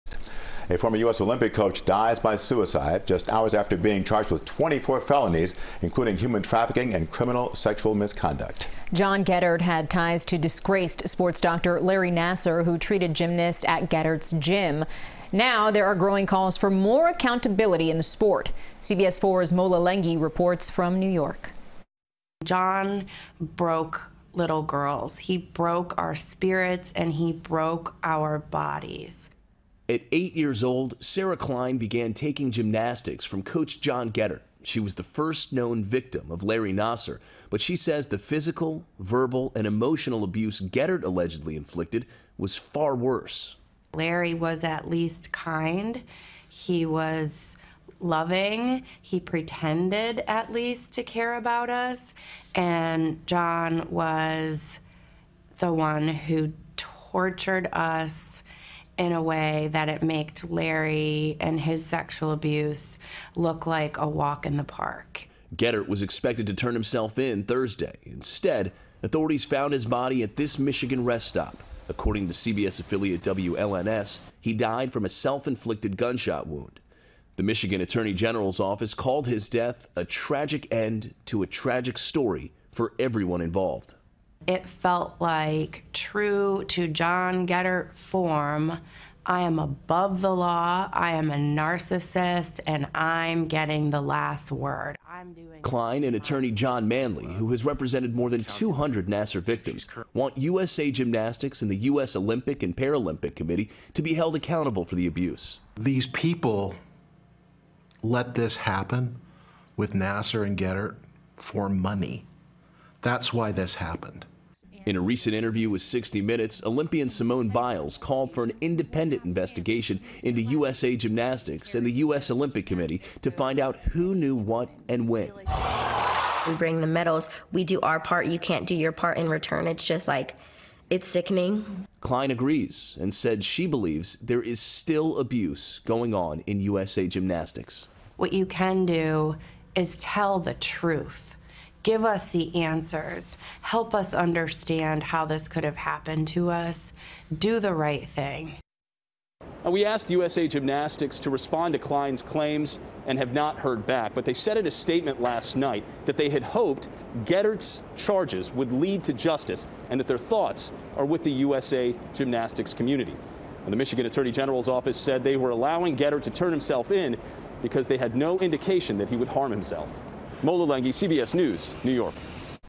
Former U.S. Olympic Coach John Geddert Dies By Suicide After Being Charged For 24 Felonies
CBS4's Mola Lenghi reports on former U.S. Olympics gymnast coach's suicide death after being charged for 24 felonies.